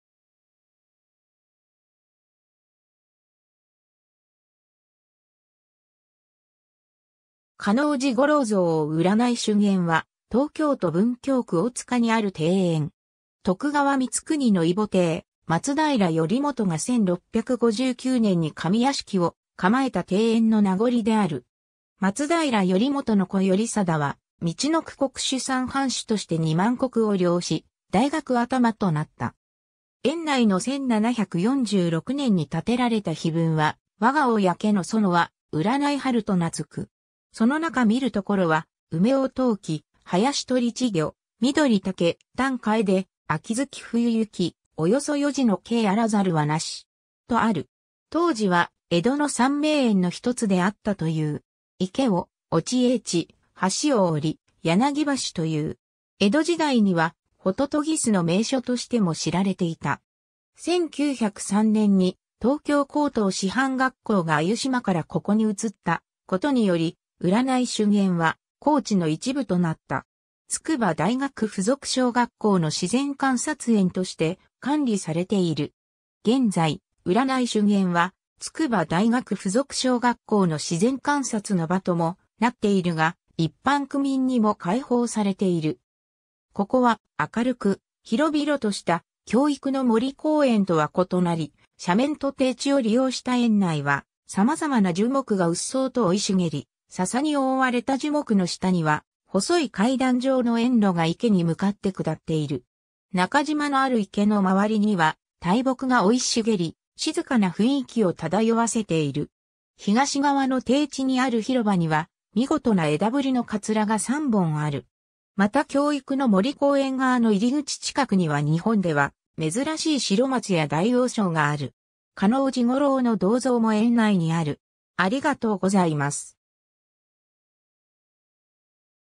占春園
嘉納治五郎像 占春園（せんしゅんえん）は東京都文京区大塚にある庭園。
徳川光圀の異母弟、松平頼元が1659年（万治2年）に上屋敷を構えた庭園の名残りである。
 松平頼元の子頼貞は陸奥国守山藩主として2万石を領し、大学頭となった。
園内の1746年（延享3年）に建てられた碑文は、「我が公の園は占春と名づく。その中見る所は、梅桜桃季、林鳥池魚、緑竹丹楓、秋月冬雪、凡そ四時の景有らざるは莫し。」とある。
当時は江戸の三名園（青山の池田邸、溜池の黒田邸）の一つであったという。
池を落英池、橋を折柳橋という。
江戸時代にはホトトギスの名所としても知られていた。
 1903年（明治36年）に東京高等師範学校（現在の筑波大学）が湯島からここに移ったことにより、占春園は校地の一部となった。
筑波大学附属小学校の自然観察園として管理されている。
 現在、占春園は、筑波大学附属小学校の自然観察の場ともなっているが、一般区民にも開放されている。
ここは、明るく広々とした教育の森公園とは異なり、斜面と低地を利用した園内は様々な樹木がうっそうと生い茂り、ササに覆われた樹木の下には細い階段状の園路が池に向かって下っている。
中島のある池の回りには大木が生い茂り、静かな雰囲気を漂わせている。
東側の低地にある広場にはみごとな枝ぶりのカツラが3本ある。
また教育の森公園側の入り口近くには日本ではめずらしいシロマツやダイオウショウがある。
嘉納治五郎の銅像も園内にある。